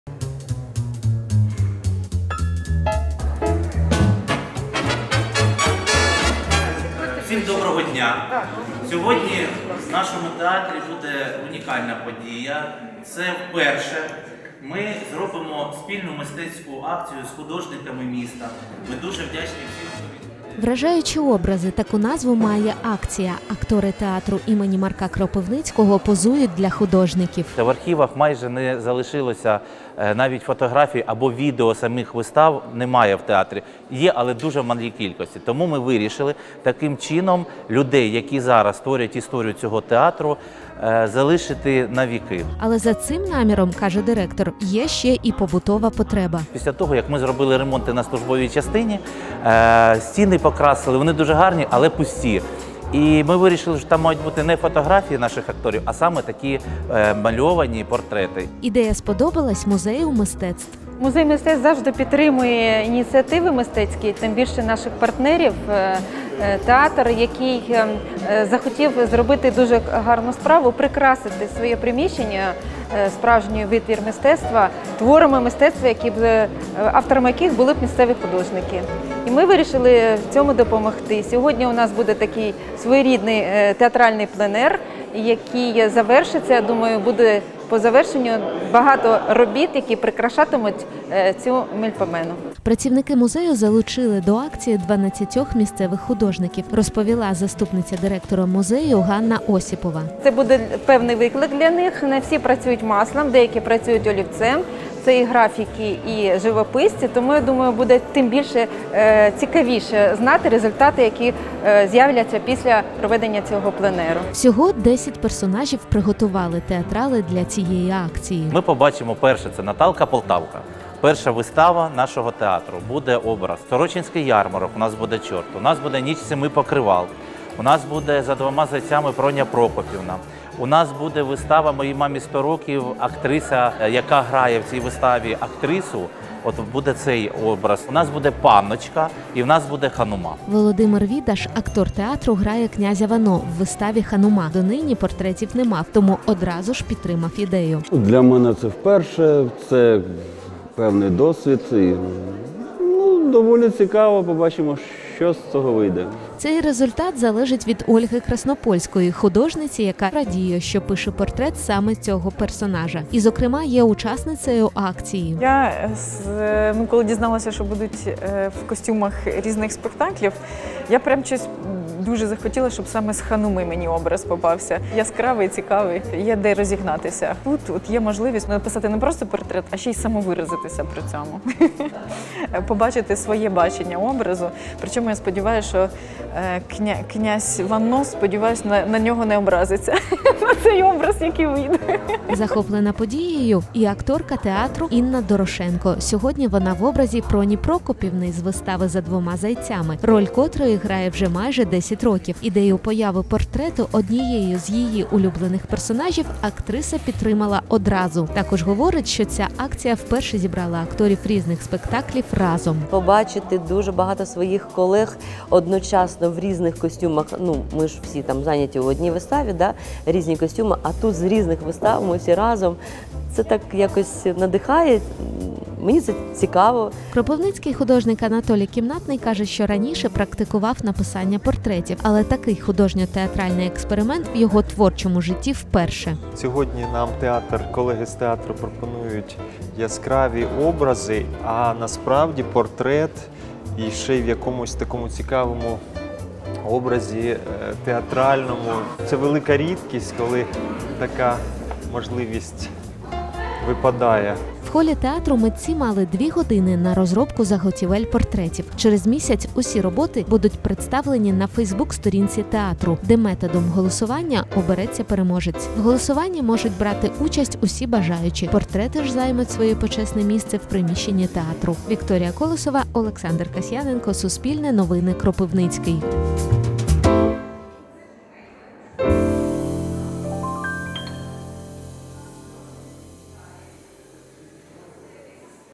Мистецьку акцію "Вражаючі образи" влаштували театр Кропивницького та Музей мистецтв
Актори театру імені Марка Кропивницького позують художникам в образах героїв спектаклів, які грають на сцені.  Це десять  найяскравіших образів з вистав з репертуару театру. Серед них: Наталк  а-Полтавка,   чорт з “Сорочинської Ярмарки”, Проня Прокіпівна з вистави “За двома зайцями”  та інші, розповів керівник театру В'ячеслав Вандрашек. Чому виникла така ідея, розпитала Вікторія Колосова.